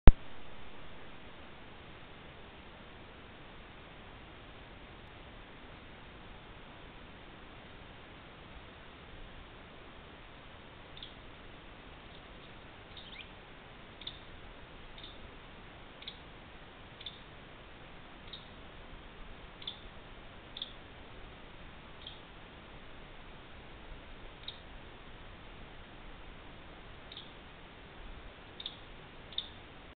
FISHER CAT
A friend I work with recorded this video on his property in Rhode Island with a wildlife cam.